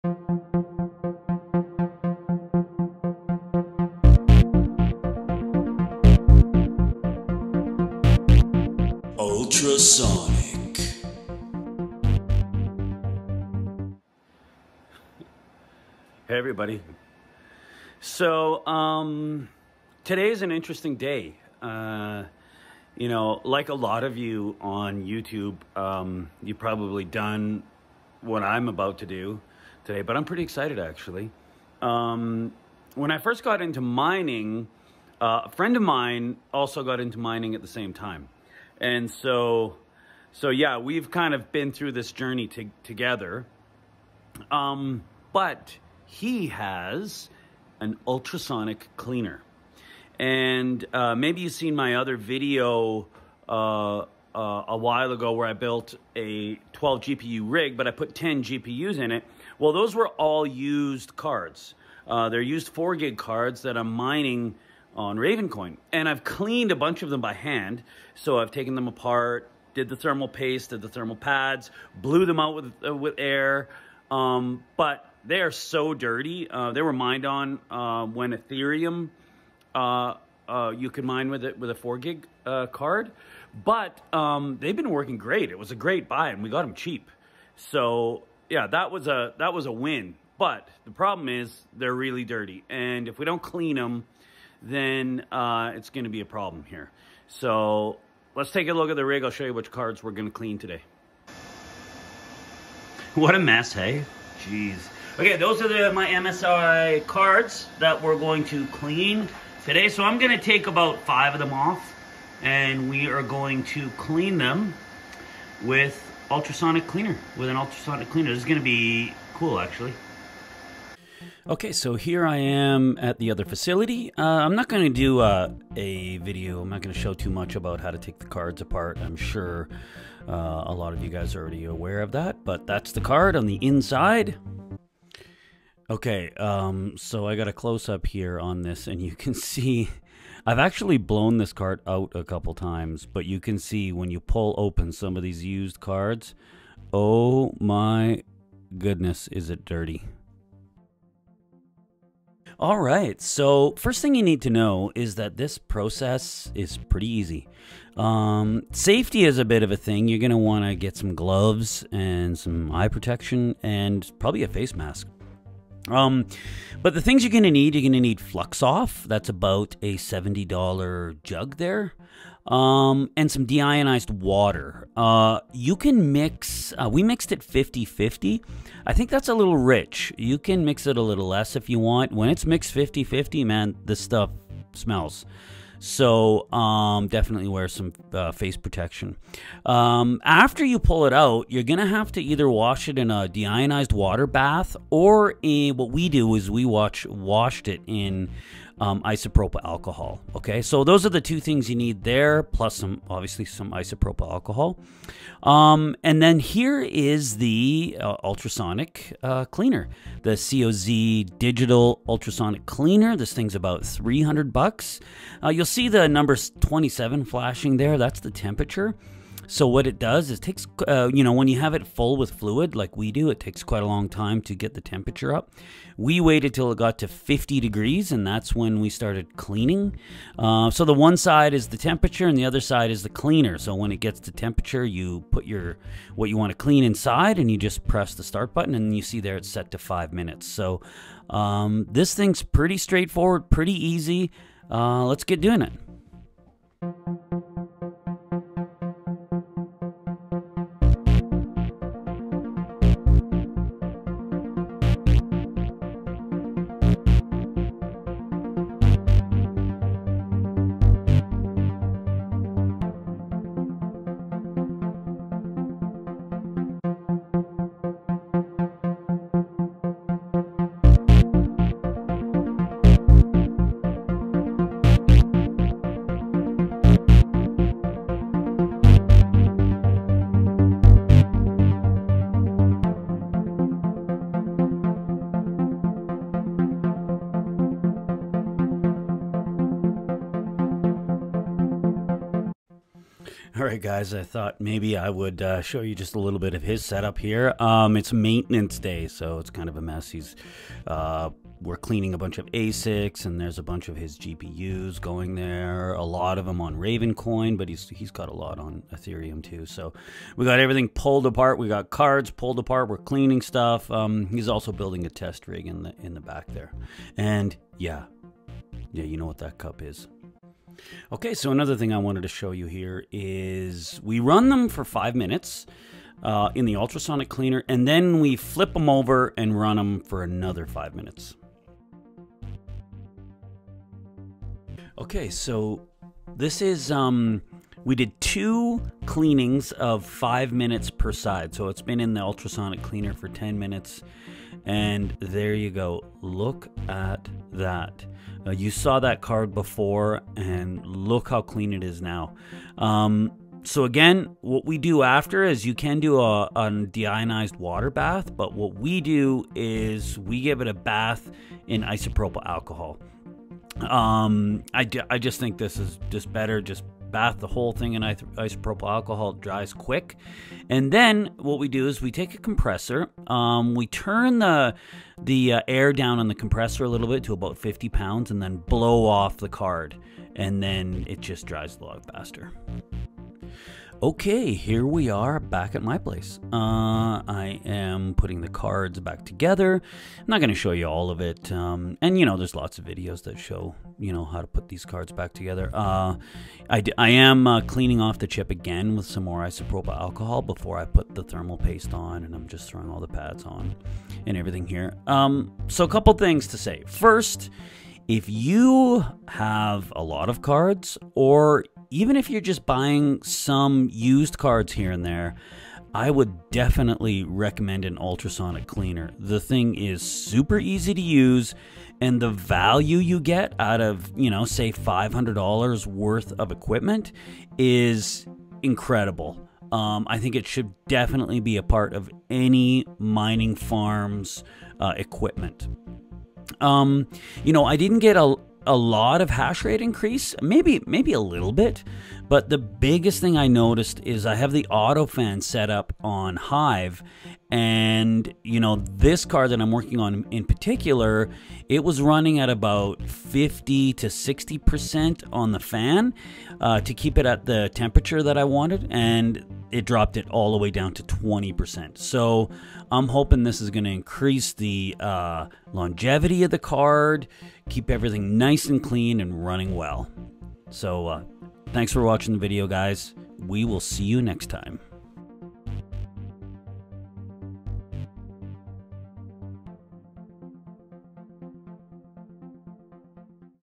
Ultrasonic Cleaning a VERY DIRTY used GPU
Are you buying used GPU's or do you have a lot of GPU's?
Then you may want to invest in a Ultrasonic Cleaner. Check out how we use ours.

For the cleaner, we bought ours of Amazon. CO-Z ultrasonic Cleaner
For the Flux Off: